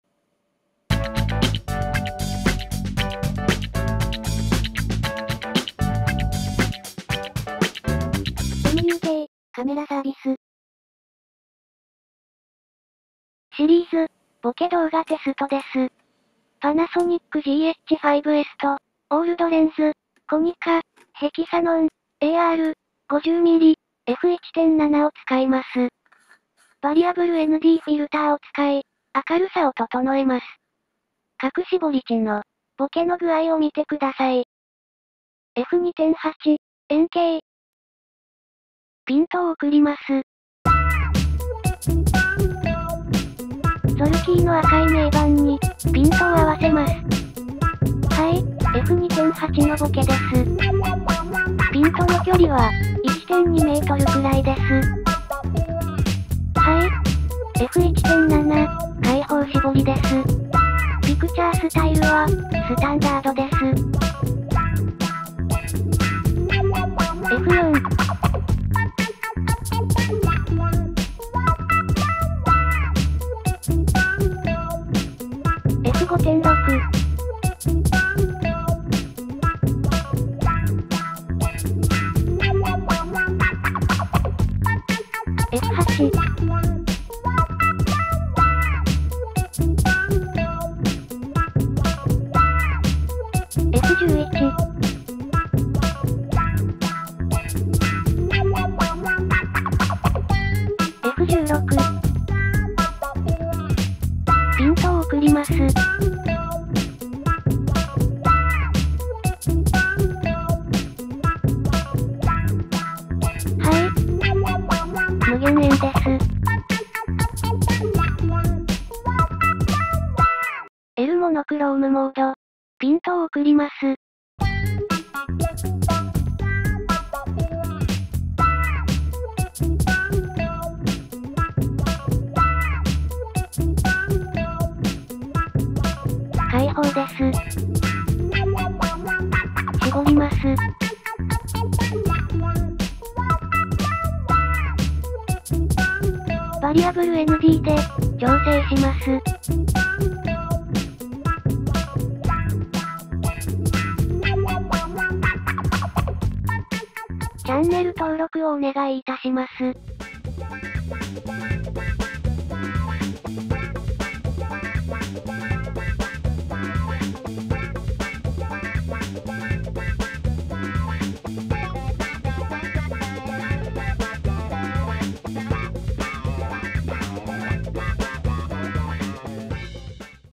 オールドレンズ Bokeh Lens Test KONICA HEXANON AR 50mm f1.7 Shot on GH5s 4K
Please try the automatic translation function of subtitles.
muk cameraservice
撮影 GX7MK2+FD28-85mm & GH5S+HEXANON AR 50mm f1.7 Bokeh Variable ND

動画編集 Sony Vegas & ACID
オールドレンズ　コニカ ヘキサノン AR 50mm f1.7 ボケテスト動画
KONICA HEXANON AR 50mm f1.7 Bokeh Movie Test
Picturestyle:Standard&L.Monochrome
4K30P
ISO 800
Variable ND